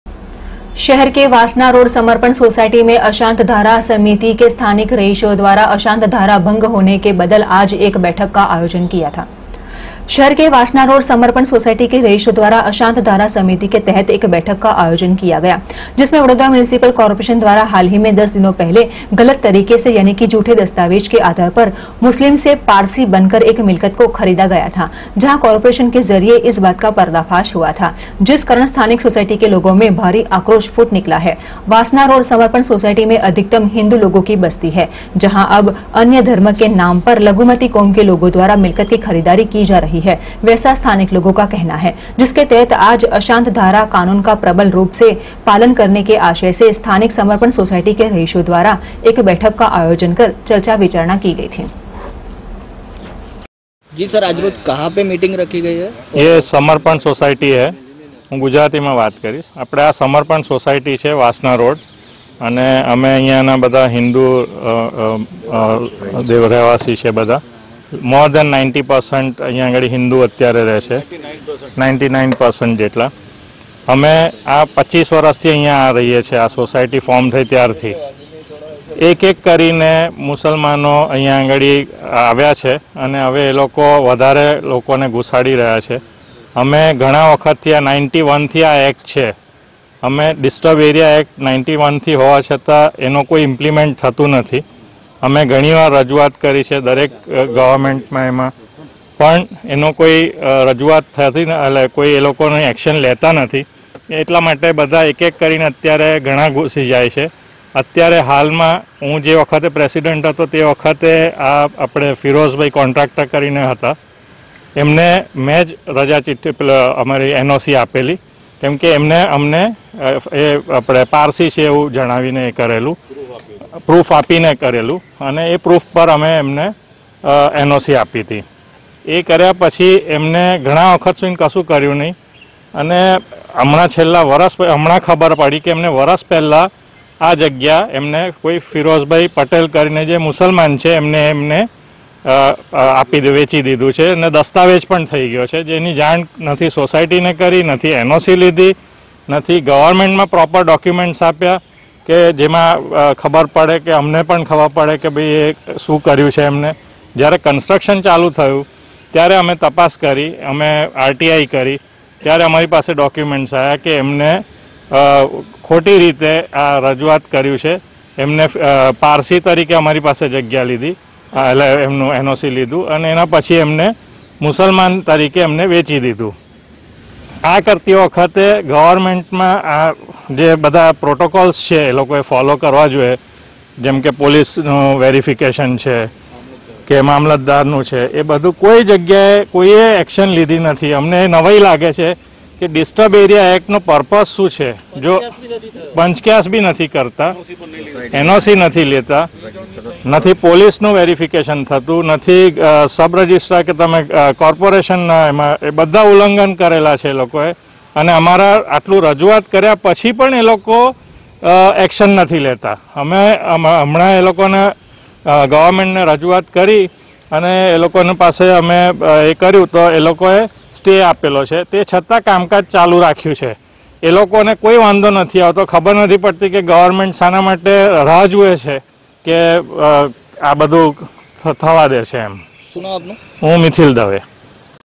Ibn news vadodara
वासना रोड समर्पण सोसाइटी में अशांत धारा समिति द्वारा  अशांत धारा भंग बदल किया गया बैठक का आयोजन